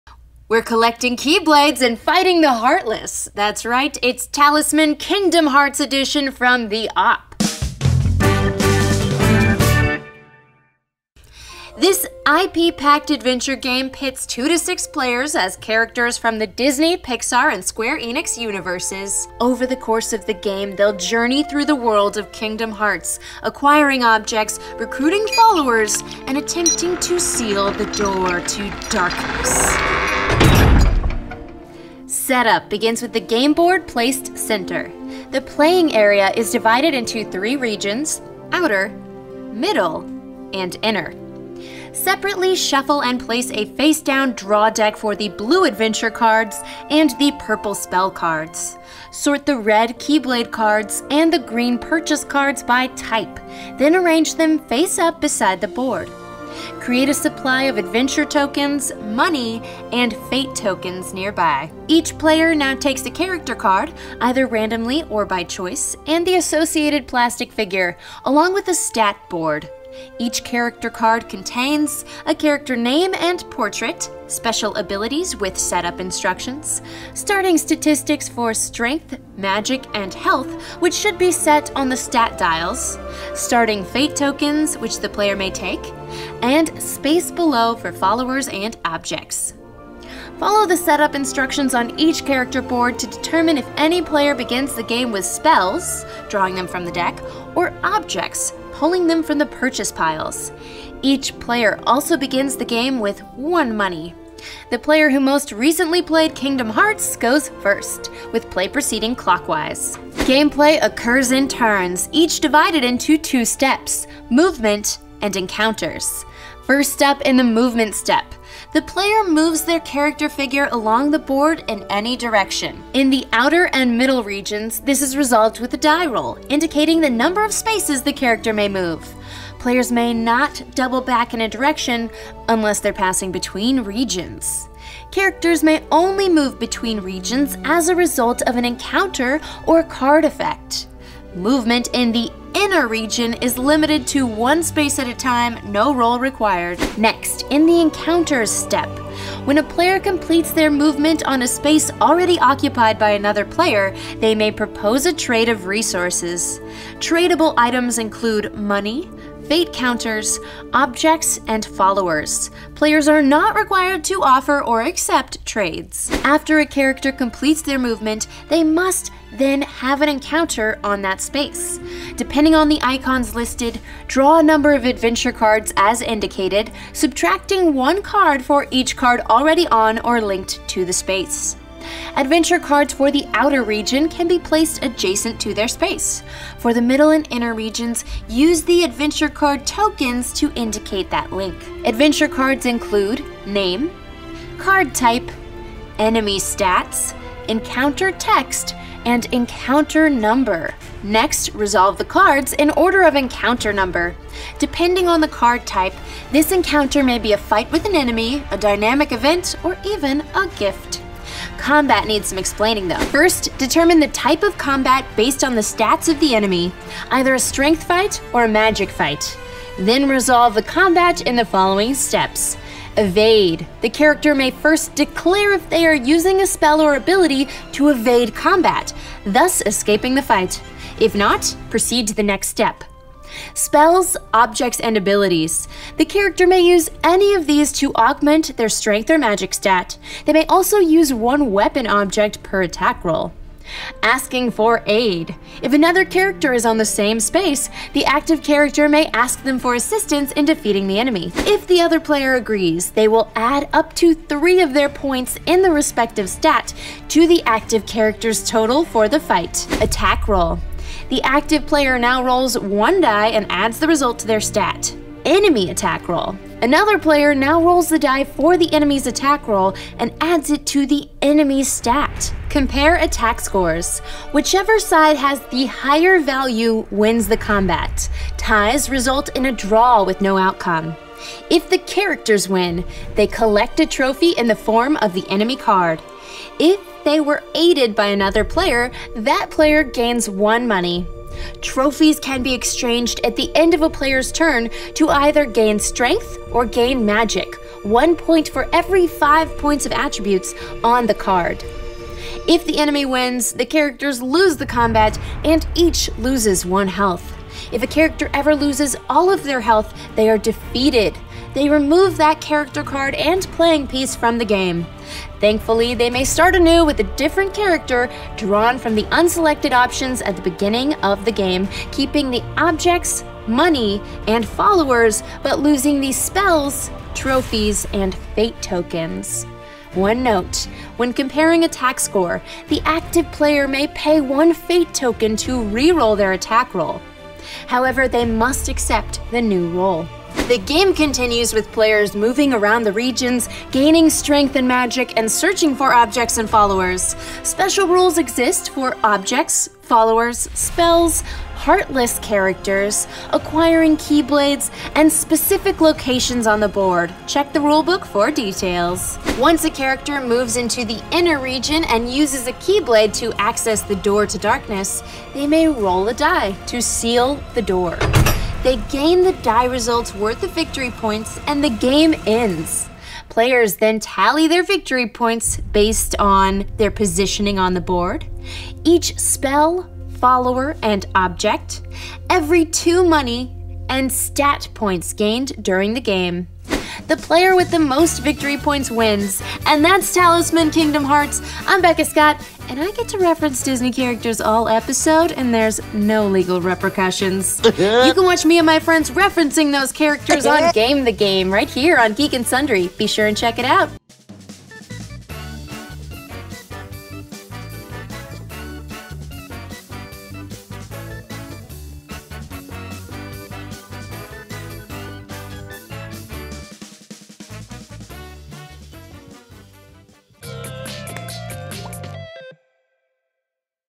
How to Play Talisman: Kingdom Hearts
Gather your friends and prepare to defeat the darkness, because in this episode of How to Play we're taking a look at Talisman: Kingdom Hearts from The OP!  You and your fellow players each assume the role of a character from the Kingdom Hearts universe to save its various worlds from impending doom.

Tabletop games are super fun, but learning the complicated directions and rules can be a challenge. Becca Scott is your game tutor, explaining the game mechanics in an easy to follow way, so you can get to playing the games faster.